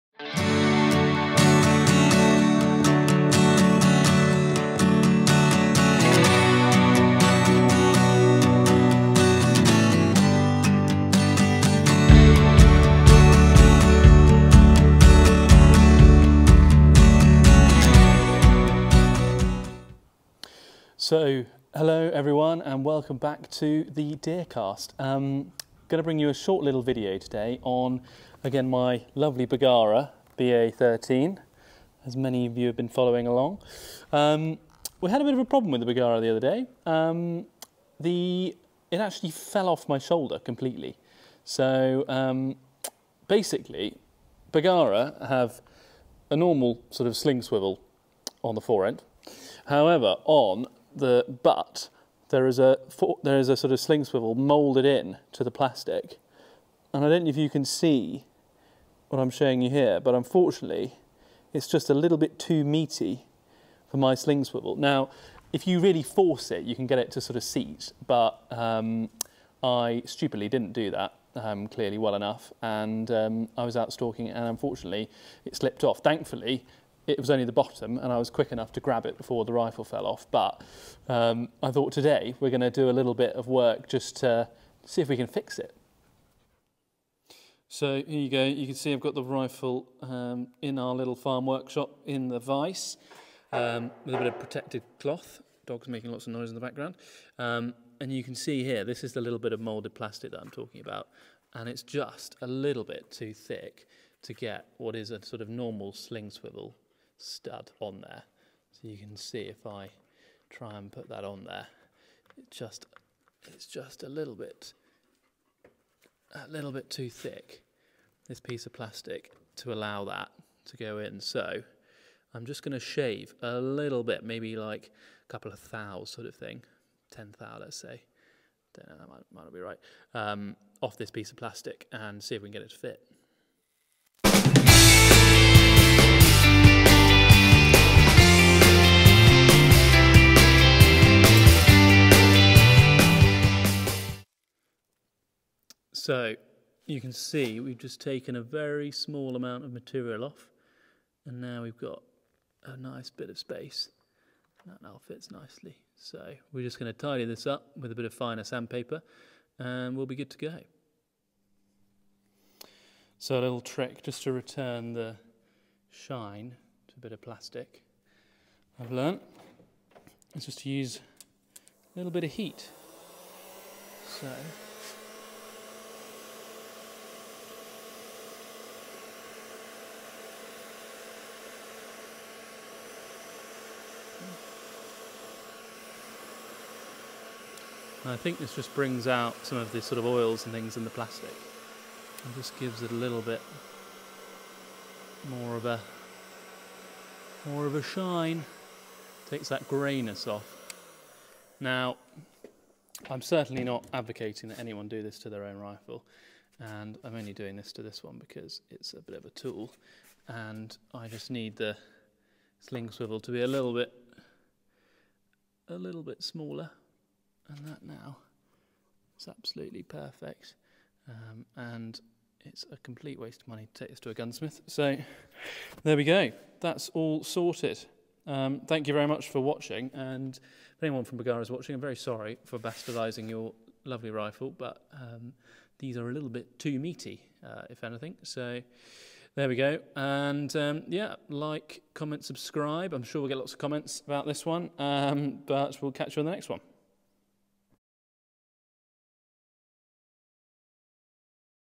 Butchering the Bergara BA13
Harry has come across a fault with the Bergara BA13 and decides to take matters into his own hands...